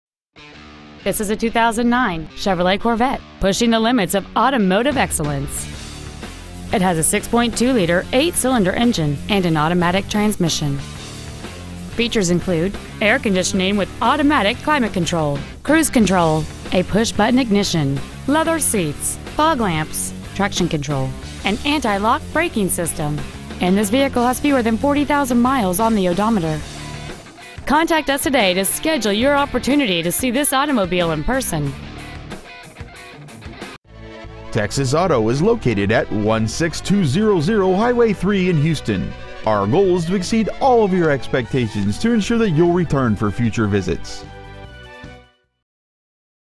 2009 Chevrolet CORVETTE C6 San Antonio TX
We are proud to present this 2009 Chevrolet CORVETTE C6 .

 Year : 2009
 Make : Chevrolet
 Model : CORVETTE C6
 Engine : 6.2L V8 SFI Engine
 Trans . : Automatic
 Exterior : Black
 Miles : 39,702
 Interior : Black

 2009 Chevrolet CORVETTE C6 Houston TX